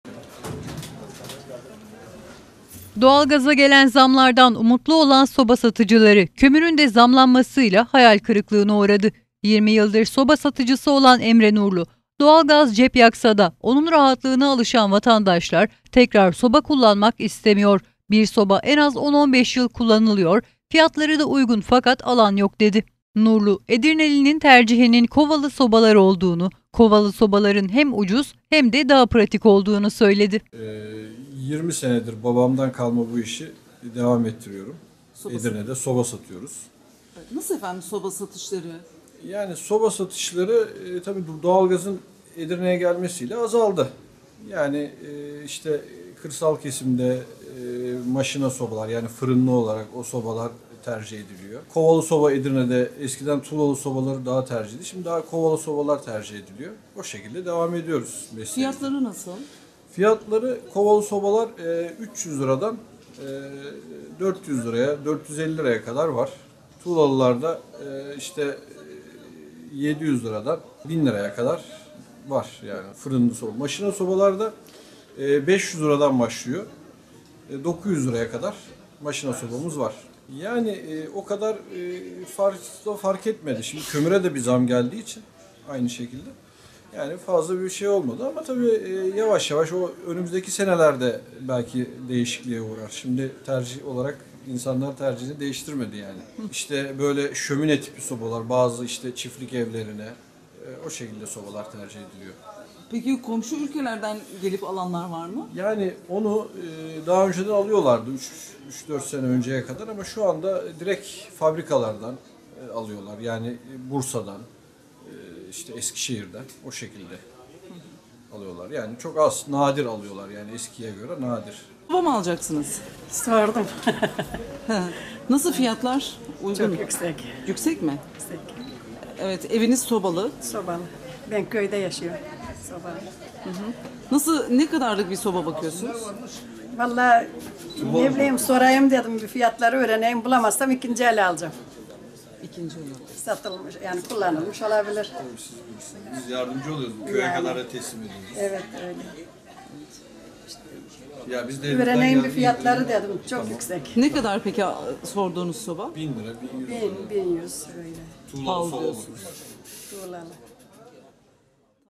SOBA FİYATLARI UCUZ AMA ALAN YOK
Doğalgaz fiyatlarının yüksek olması soba satıcılarını umutlandırıyor. Edirne’ye doğalgaz gelmesi ile birlikte soba satışlarının azaldığını söyleyen esnaf Emre Nurlu, doğalgaz fiyatının zamlanması ile birlikte “önümüzdeki yıllarda eskiye dönüş olacak, vatandaş soba kullanmaya başlayacak” dedi.